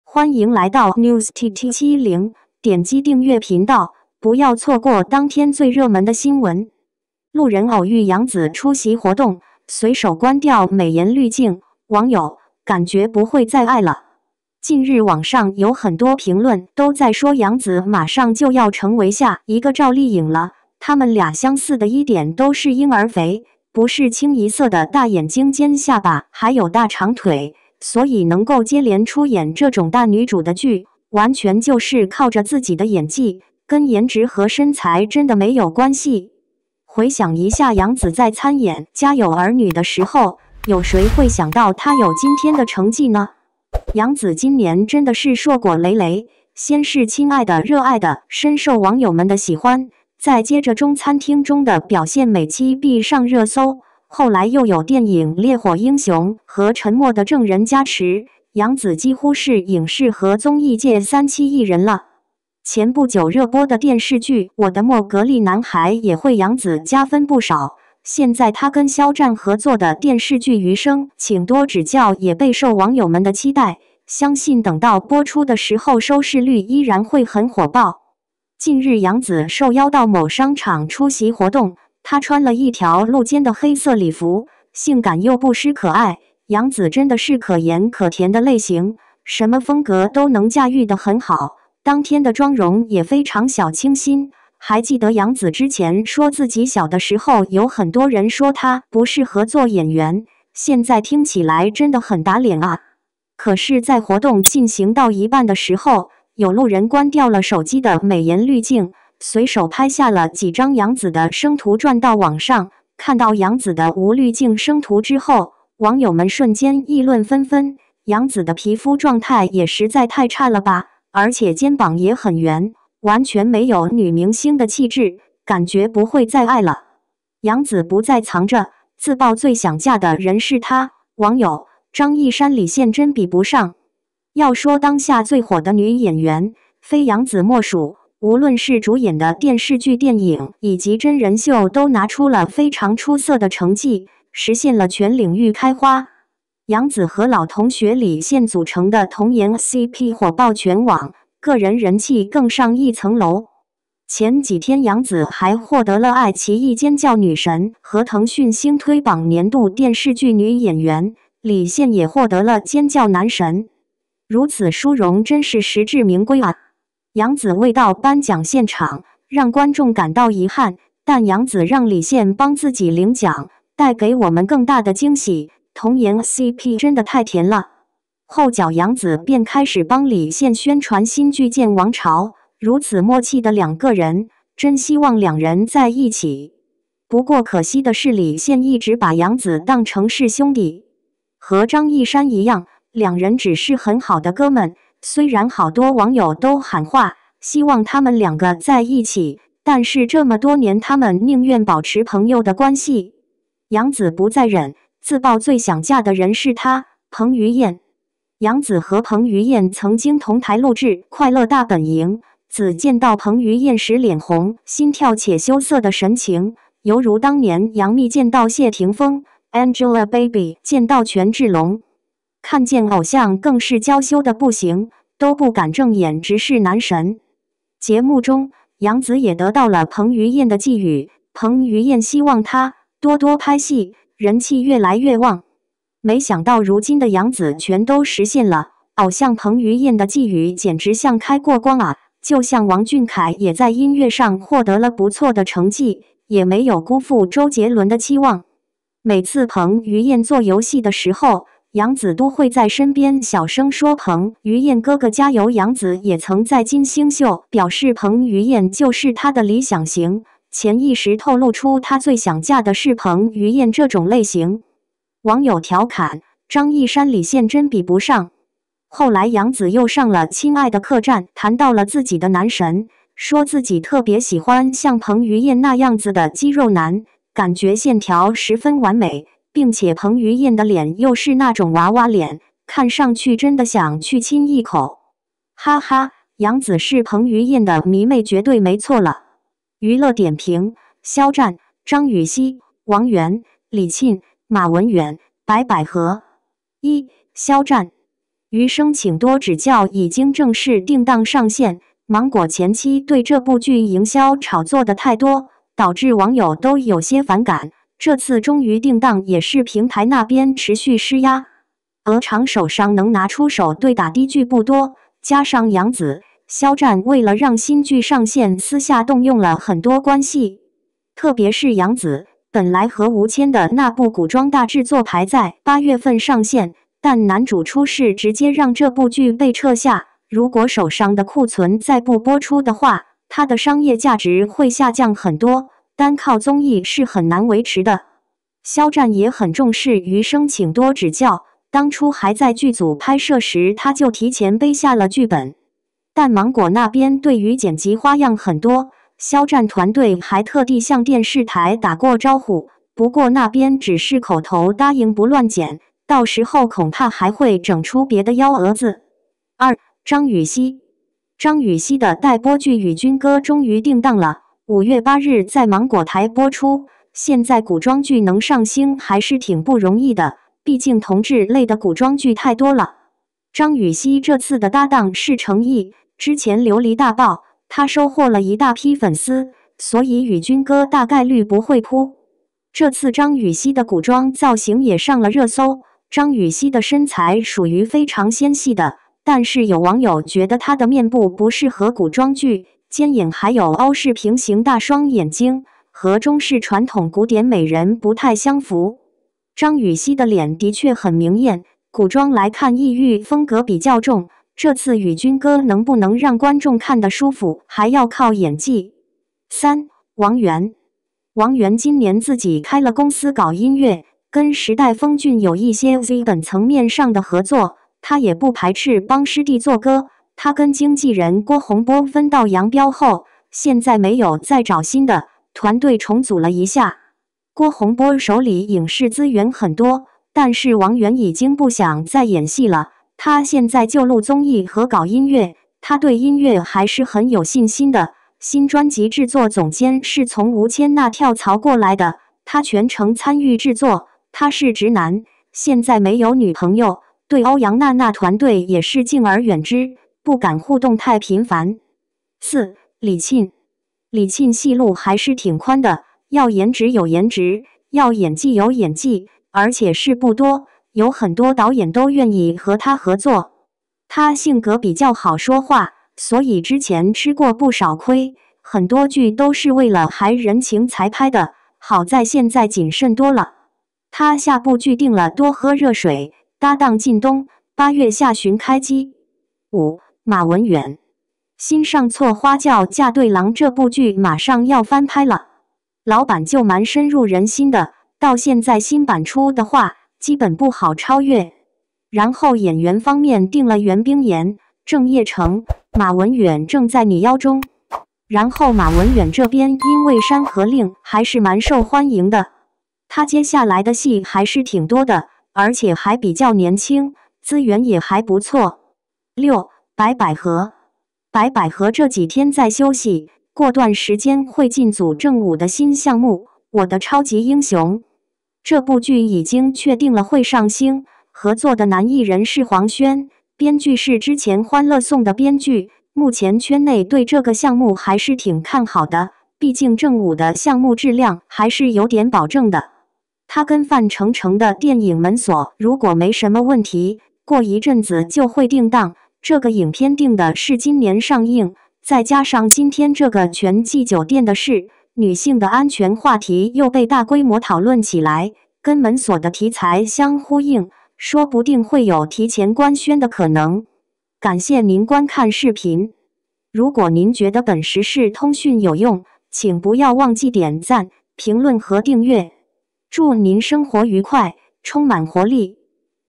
ENG SUB -路人偶遇#杨紫 出席活动，随手关掉美颜滤镜，杨紫 不再藏着，自爆最想嫁的人是他 【NEWS TT7007月09日】
路人偶遇#杨紫 出席活动，随手关掉美颜滤镜，杨紫 不再藏着，自爆最想嫁的人是他
娱乐点评：肖战、张予曦、王源、李沁、马闻远、白百何